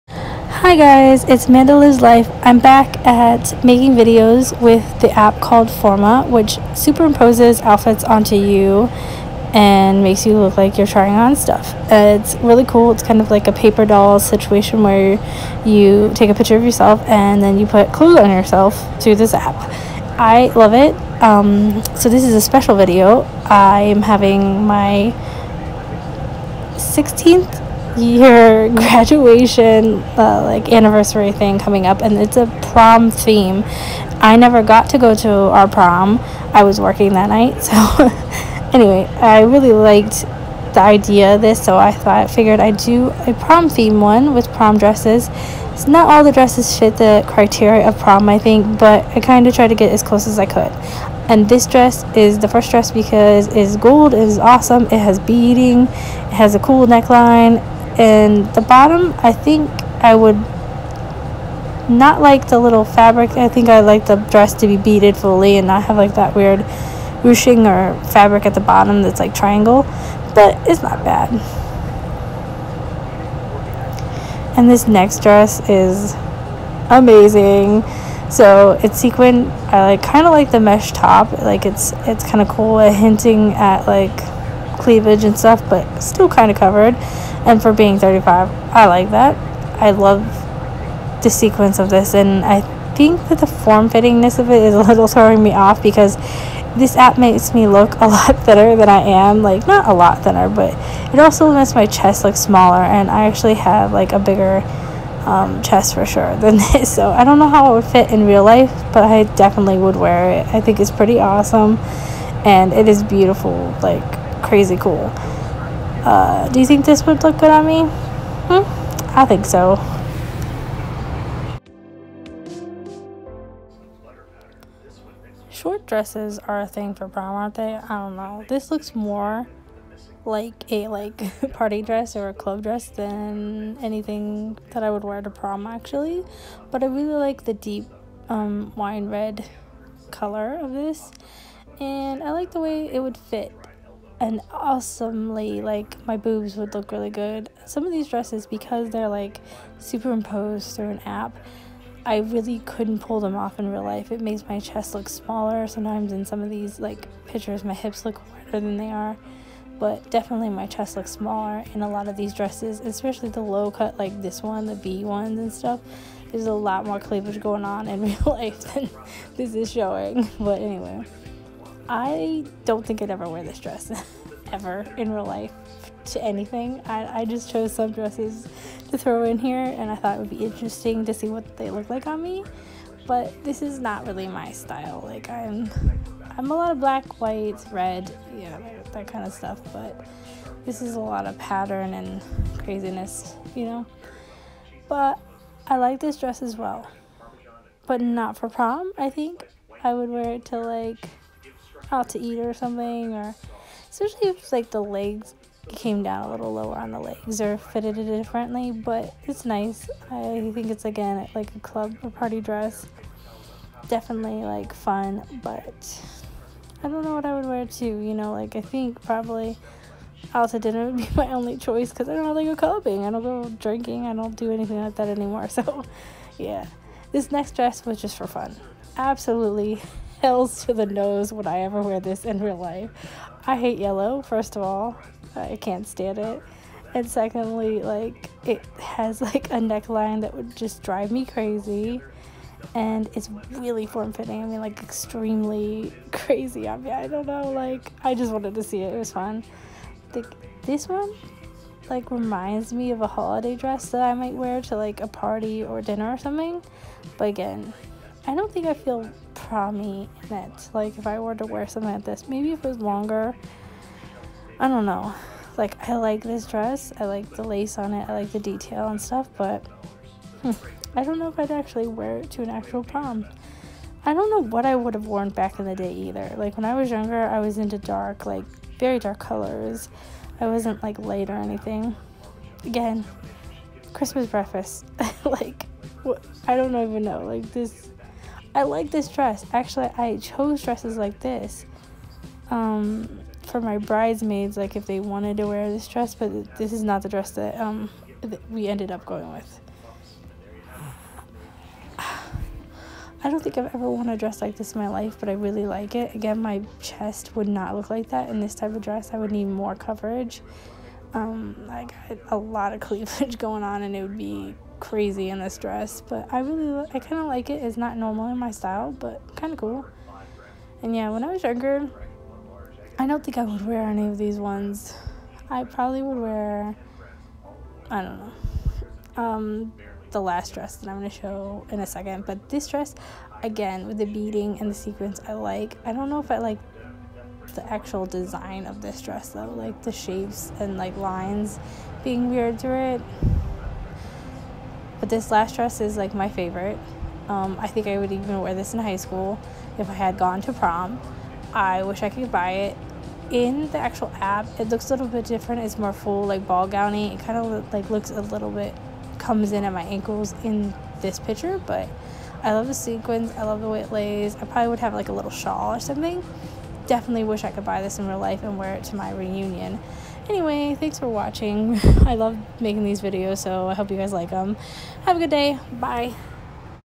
Plus Size Try On With. Forma App Prom Dress Edition
Hi guys. Madeline’s life here. Today I’m using my favorite app to try on dresses for prom. I never went to Parma but I have a 16 year high school Reunion coming out. This reunion is prom theme. I’m really excited I would like to go. Actually I’m planning on going if we can.